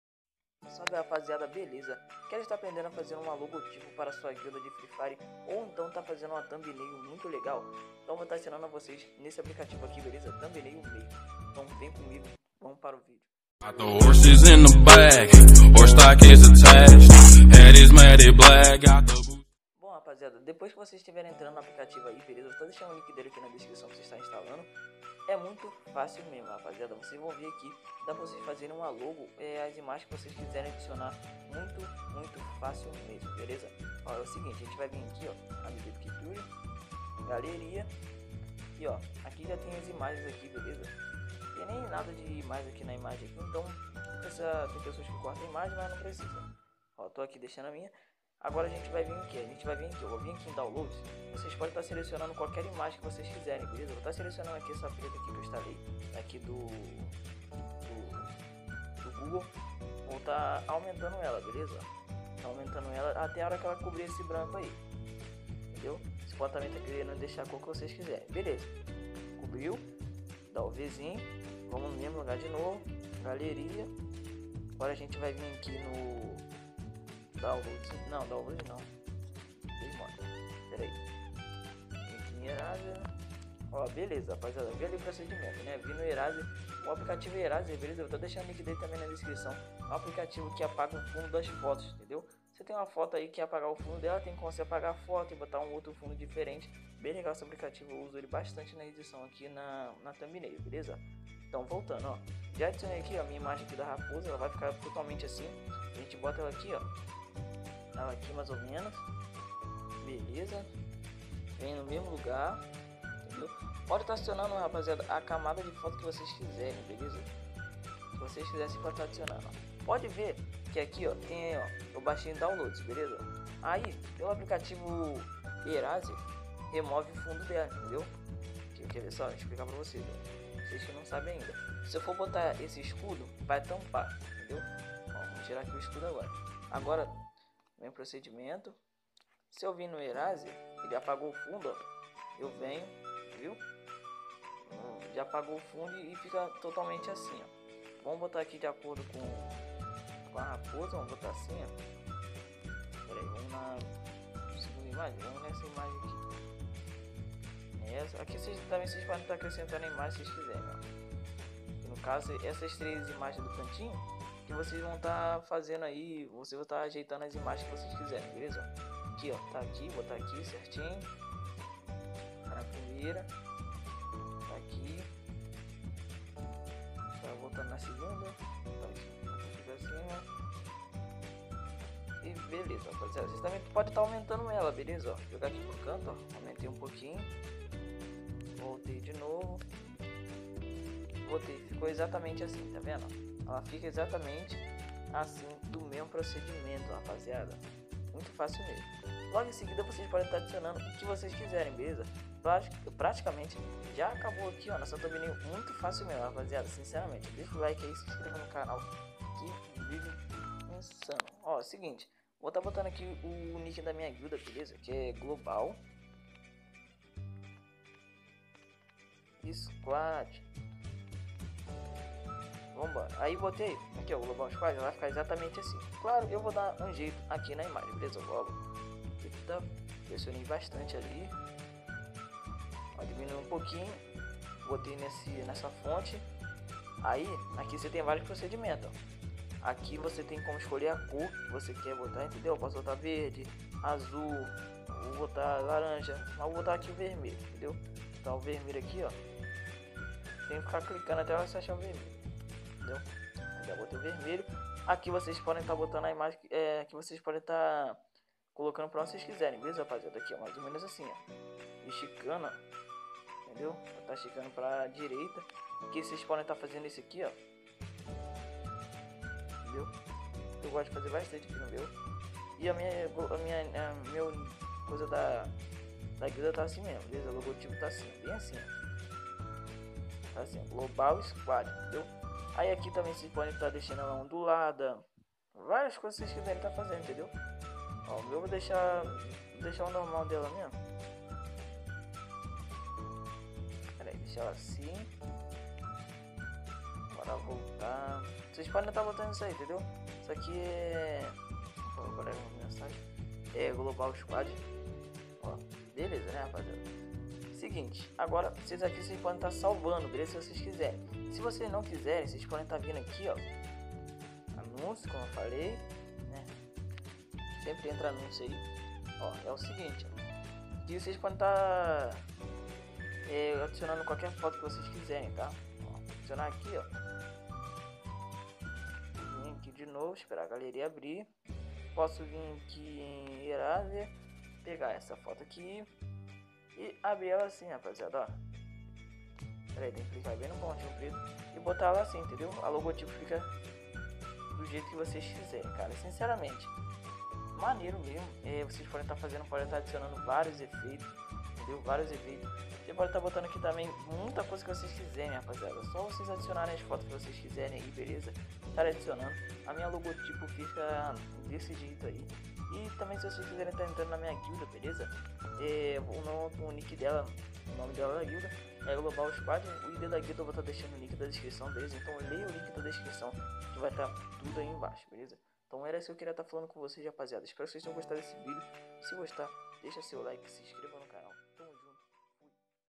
COMO FAZER UMA LOGOTIPO RÁPIDO E FÁCIL !! ( B.E Gamer )
Hj é segunda e é dia de vídeo novoooo..

APRENDA A FAZER UMA LOGOTIPO PARA A SUA GUILDA OU QUALQUER ALGO DO TIPO ( COMO THUMBNAILS E TALS )

Também me siga no insta😝: @BRUUNINHOOYT √

COMPARTILHE..

FUI ABRAÇO..